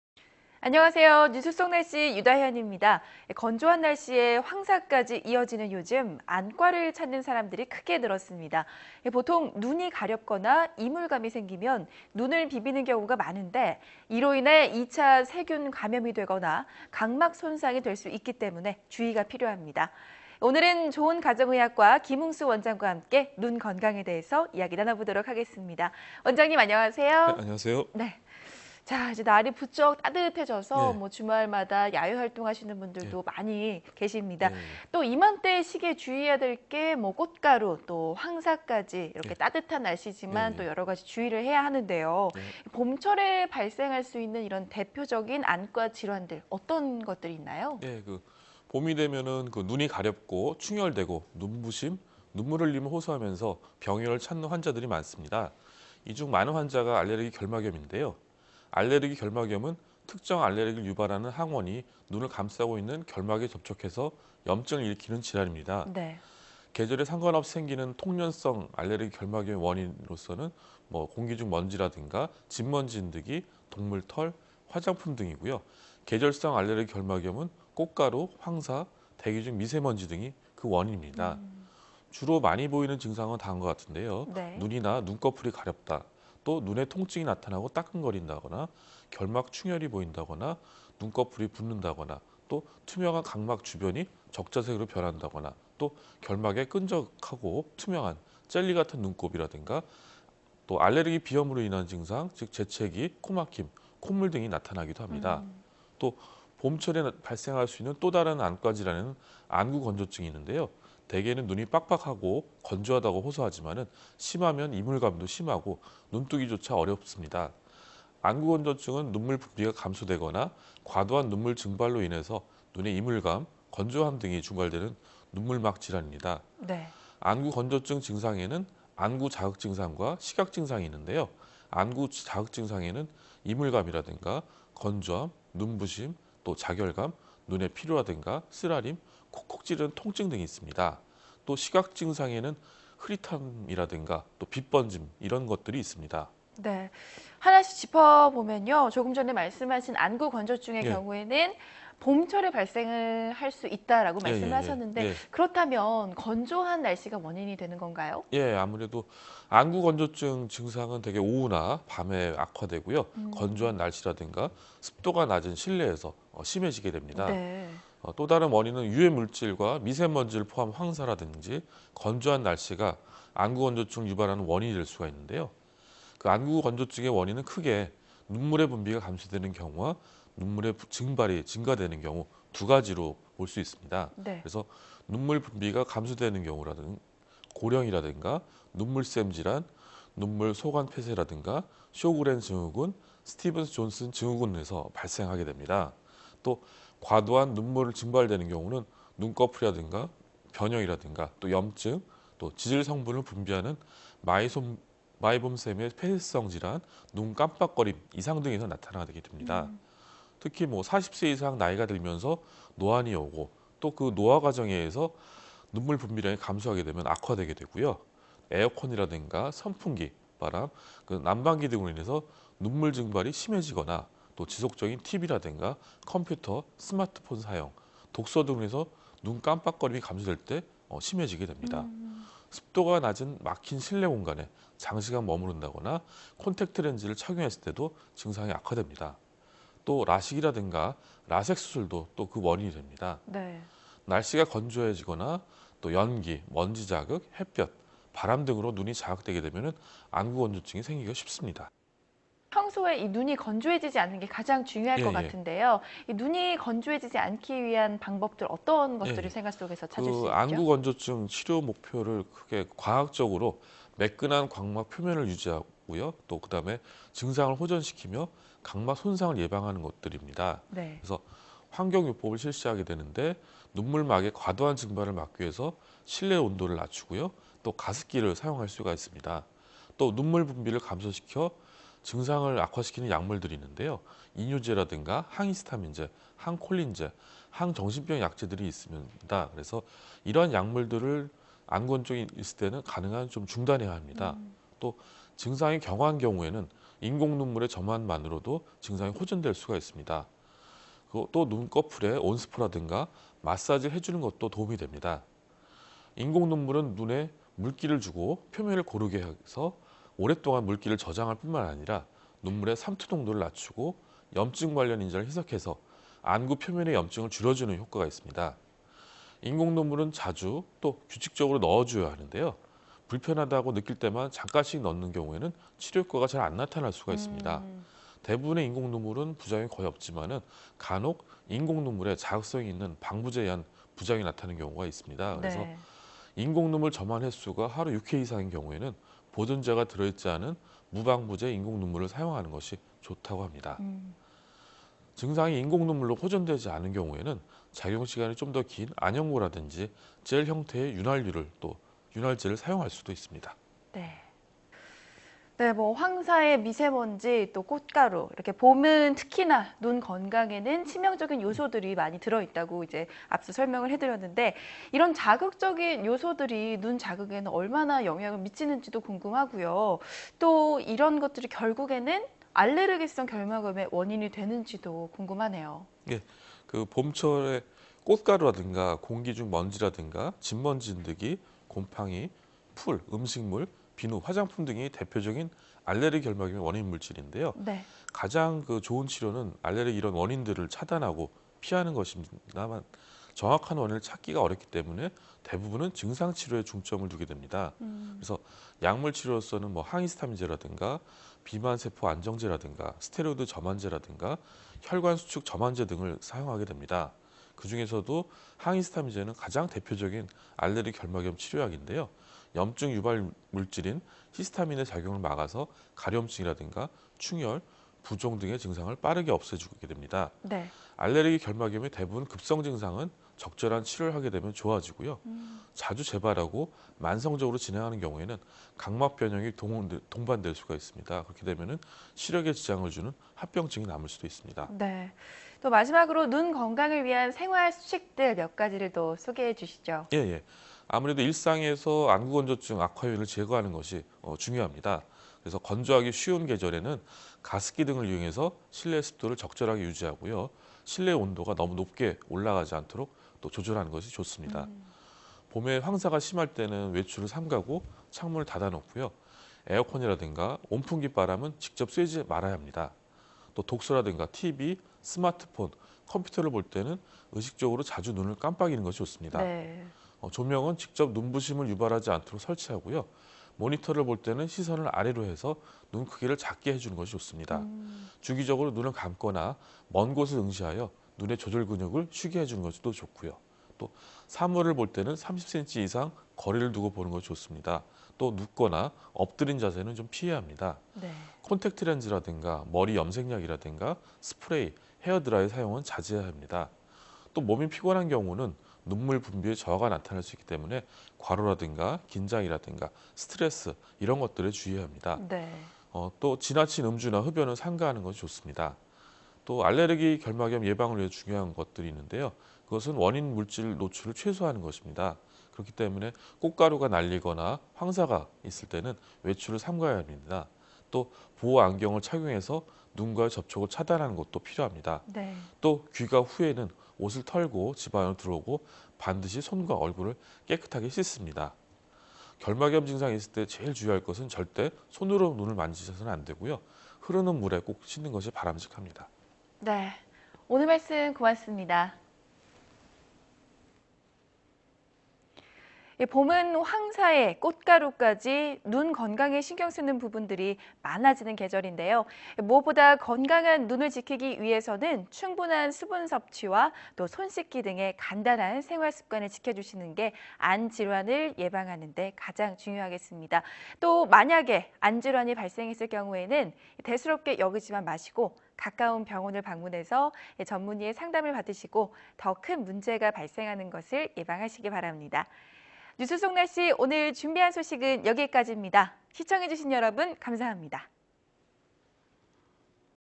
충혈되고 뻑뻑한 '눈'··· 안질환 예방법 [03/13] / YTN 웨더
안녕하세요. 뉴스 속 날씨 유다현입니다.

건조한 날씨에 황사까지 이어지는 요즘 안과를 찾는 사람이 크게 늘었는데요.

보통 눈이 가렵거나 이물감이 생기면 눈을 비비는 경우가 많은데, 이로 인해 2차 세균 감염이 되거나 각막 손상이 될 수 있기 때문에 주의가 필요합니다.

오늘은 좋은가정의학과 김응수 원장과 함께 눈 건강에 대해 이야기 나눠보겠습니다.

원장님, 안녕하세요.

●질문 1 : 날이 부쩍 따뜻해졌습니다.
주말마다 야외활동하시는 분들도 부쩍 늘었는데요. 이맘때 시기에는 꽃가루에 황사까지 눈 건강에 치명적인 것들이 많죠. 봄철 발생할 수 있는 대표적 안과 질환, 어떤 것들이 있습니까?

●답변 1 : 봄이 되면, 눈이 가렵고 충혈되고 눈부심, 눈물 흘림을 호소하며, 병의원을 찾는 환자들이 많습니다. 이 중 많은 환자가 알레르기 결막염입니다. 알레르기 결막염은 특정 알레르기를 유발하는 항원이 눈을 감싸고 있는 결막에 접촉해 염증을 일으키는 질환입니다. 계절에 상관없이 생기는 통년성 알레르기 결막염의 원인은 미세먼지, 집먼지 진드기, 동물 털, 화장품 등이고 계절성 알레르기 결막염은 꽃가루와 황사, 대기 중 미세먼지 등이 원인입니다.
주로 많이 보이는 증상은 다음과 같습니다. 눈이나 눈꺼풀이 가렵다. 눈의 통증이 나타나고 따끔거린다. 결막 충혈이 보인다. 눈꺼풀이 붓는다. 투명한 각막의 주변이 적자색으로 변한다. 결막에 끈적하고 투명한 젤리 같은 눈꼽이 생긴다. 알레르기 비염으로 인한 재채기, 코막힘, 콧물 등이 나타나기도 합니다.
봄철에 발생할 수 있는 또 다른 안과 질환에는 안구건조증이 있습니다. 대개는 눈이 뻑뻑하고 건조하다고 호소하지만 심하면 이물감도 심하고 눈뜨기 어려워합니다. 안구건조증은 눈물 분비가 감소되거나 과도한 눈물 증발로 인해 눈에 이물감, 건조함 등이 유발되는 눈물막 질환입니다. 안구건조증 증상에는 안구 자극증상과 시각증상이 있습니다. 안구 자극증상에는 이물감, 건조함, 눈부심, 작열감, 눈의 피로, 쓰라림, 콕콕 찌르는 통증 등이 있습니다. 시각증상에는 흐릿함, 빛 번짐 등이 있습니다.

●질문 2 : 하나씩 짚어보도록 하겠습니다. 조금 전 말씀하신 안구건조증의 경우에 봄철에 발생할 수 있다고 하셨는데요. 건조해진 날씨가 원인인 건가요?

●답변 2 : 안구건조증 증상들은 오후나 밤에 악화되고, 건조한 날...